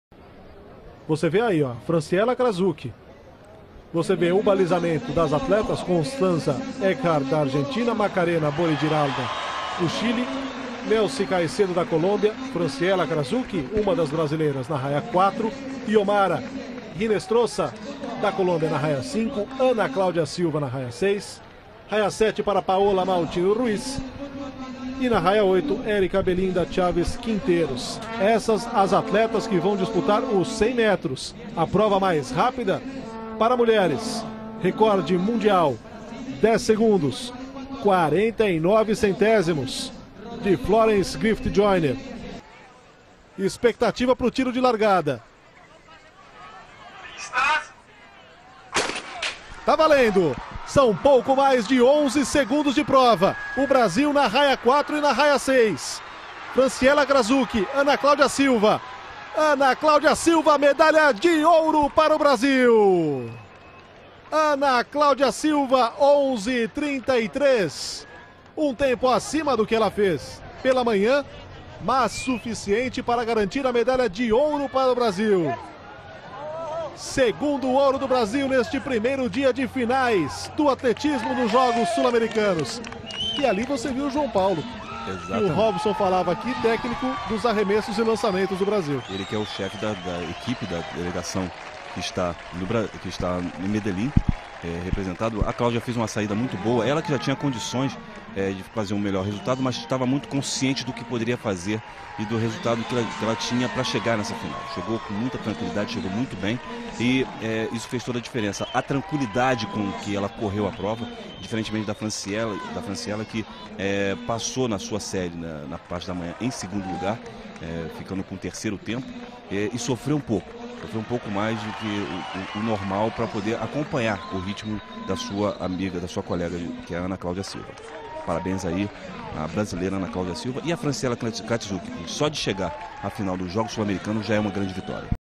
Medellín 2010 - Final dos 100m femininos.
Jogos Sulamericanos Medellín 2010

Final dos 100 metros rasos femininos:

Ana Claudia Silva (BRA) - 11.33

Yomara Hinestroza (COL) - 11.63

Nelcy Caicedo (COL) - 11.70